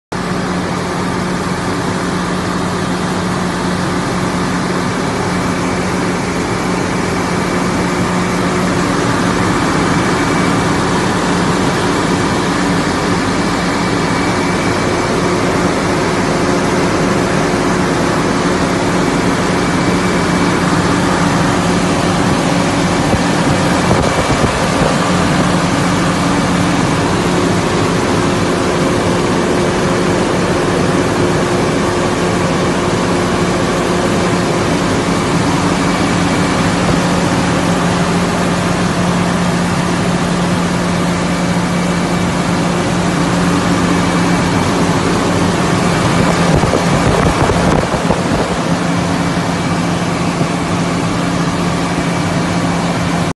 2017 CATERPILLAR 330D2L For Sale
330D2L Caterpillar Excavator.
Serial No: XDR10006.
Year: 2017.
Hours: 8,332.
Engine: CAT C7.1 ACERT - 213hp
Bucket: 2.10 Cubic Yards.
Tracks: 600mm.
Piped For Hammer.
Dims: L X W X H - 10,360 x 3,190 x 3,330mm